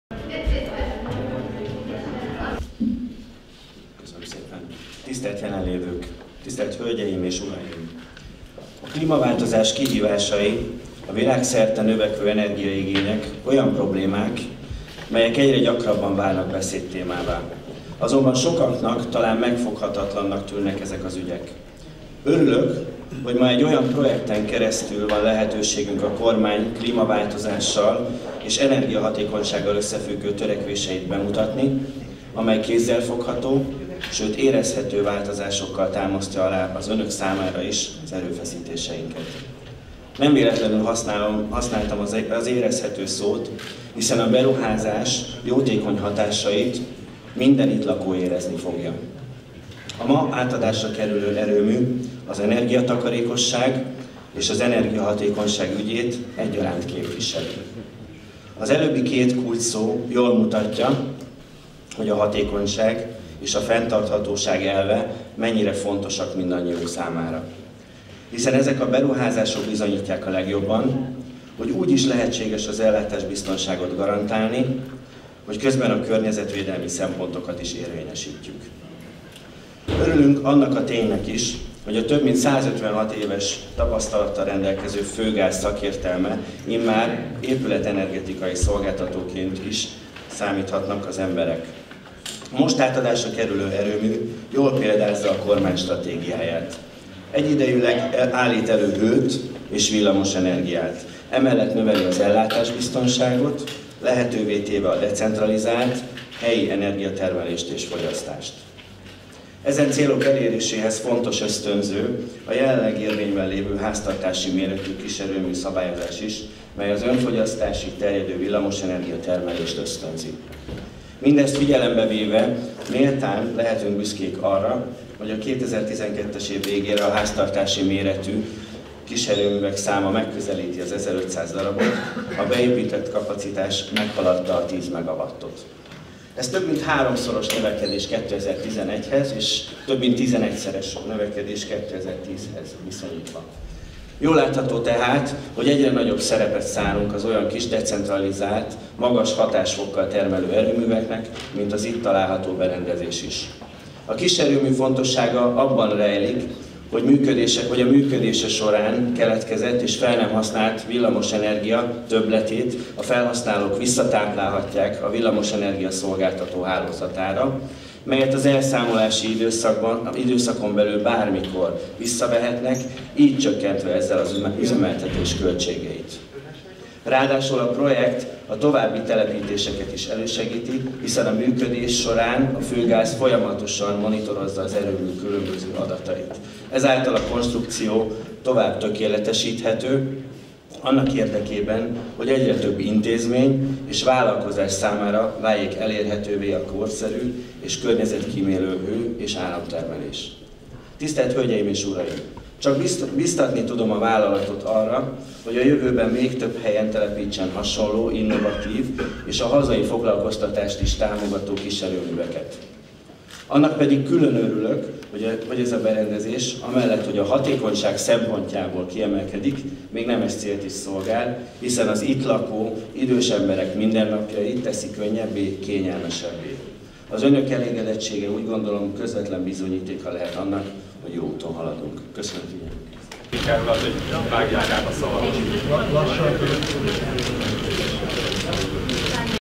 Átadta első háztartási kiserőművét a Főgáz
Mintegy 20 millió forintba került az a gázmotoros kis erőmű, amelyet a Főgáz telepített az Ingatlanbank tulajdonában lévő idősek otthonába Budapesten, a megtérülés 15 év alatt várható.